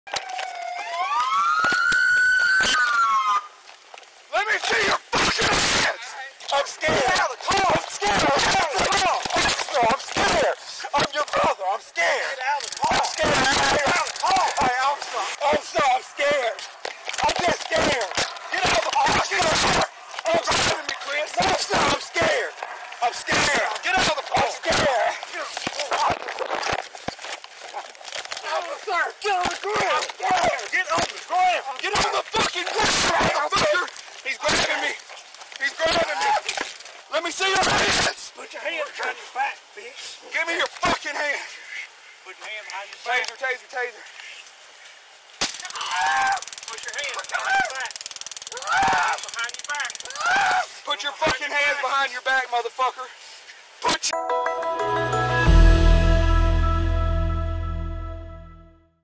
İki Yıl Sonra Ortaya Çıkan Video Yeni Polis Şiddeti Tartışması Başlattı
AP’nin yayınladığı polis videosunda 10 Mayıs 2019’da aracı bir ağaca çarpan siyah sürücü Ronald Green’in polis tarafından el ve ayaklarından kelepçeli halde zorla yüzüstü yatırıldığı, nefes alabilmek için dönmeye çabalayınca da polis tarafından engellendiği görülüyor.  İki yıldır örtbas edilen olayl